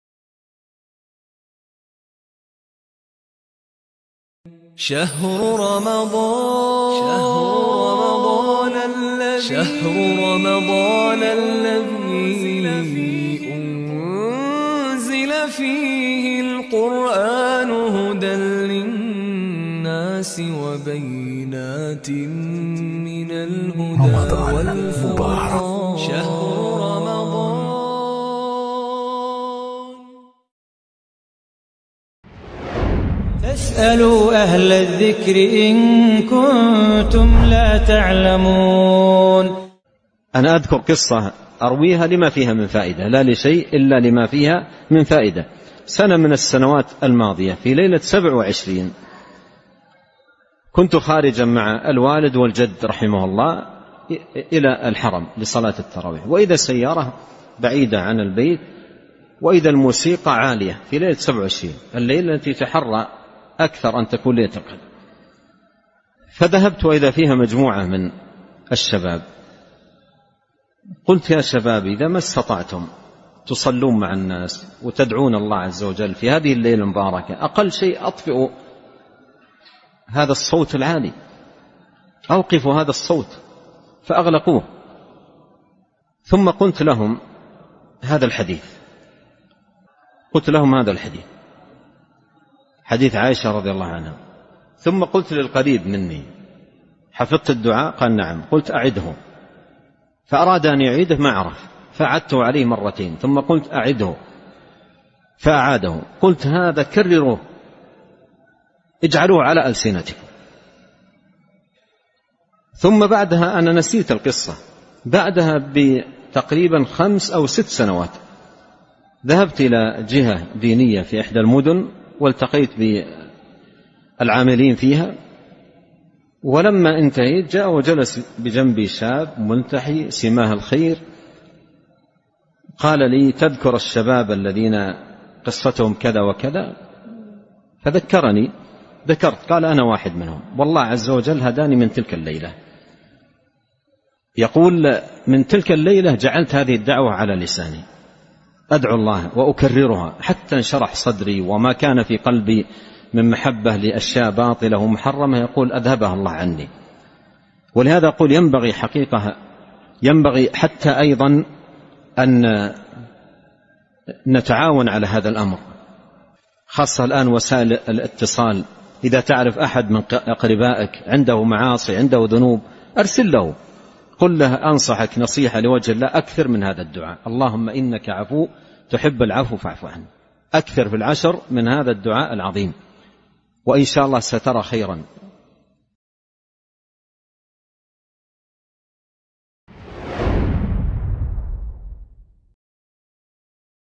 Kisah Para Pemuda di Malam 27 Ramadhan
Kisah insipratif tentang para pemuda di suatu malam 27 ramadhan.

Kisah ini diceritakan oleh Syaikh Abdurrozaq bin ‘Abdul Muhsin al-‘Abbâd.
Sebuah kisah yang semoga bisa menjadi penyemangat untuk kita untuk terus optimis atas rahmat Allah.

Video ini diambil dari channel Shahih Fiqih. Semoga menjadi ladang pahala untuk para asatidz dan seluruh team Shahih Fiqih

Semoga bermanfaat. Barakallahu Fiikum